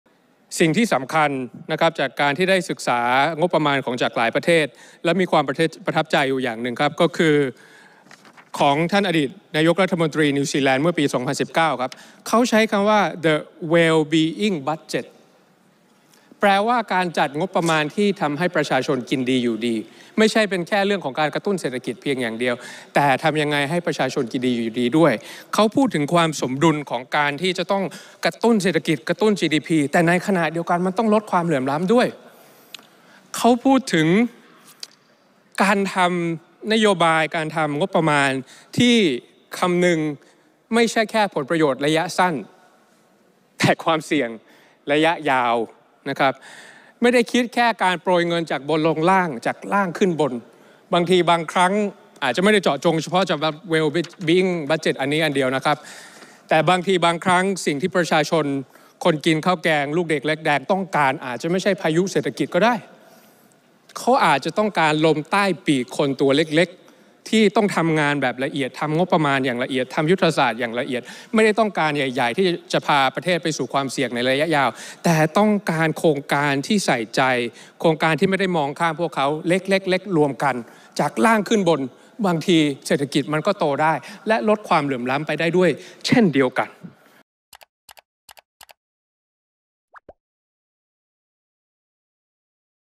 'พิธา' : งบที่ประชาชนกินดีอยู่ดี ต้องลดความเหลื่อมล้ำด้วย | 21 มิ.ย. 67
พิธา ลิ้มเจริญรัตน์ สส. บัญชีรายชื่อ พรรคก้าวไกล อภิปรายงบฯ 68 คนสุดท้ายของฝ่ายค้าน ในการประชุมสภาผู้แทนราษฎร วาระพิจารณาพระราชบัญญัติงบประมาณรายจ่ายประจําปีงบประมาณ พ.ศ. 2568 กล่าวว่า ได้ศึกษาการจัดทำงบประมาณของหลายประเทศ ประทับใจของ อดีตนายกฯ นิวซีแลนด์เมื่อปี 2019 ที่ใช้คำว่า The Wellbeing Budget หรือ 'การจัดงบที่ทำให้ประชาชนกินดีอยู่ดี' ที่พูดถึงการกระตุ้นเศรษฐกิจกระตุ้น GDP ขณะเดียวกันก็ต้องลดความเหลื่อมล้ำด้วย
“บางครั้งสิ่งที่ประชาชนคนเดินดินกินข้าวแกงต้องการ อาจไม่ใช่พายุหมุนทางเศรษฐกิจ แต่ต้องการลมใต้ปีกให้คนตัวเล็กๆ ผ่านการทำงบประมาณและยุทธศาสตร์อย่างละเอียด มีโครงการที่ใส่ใจ” พิธา กล่าว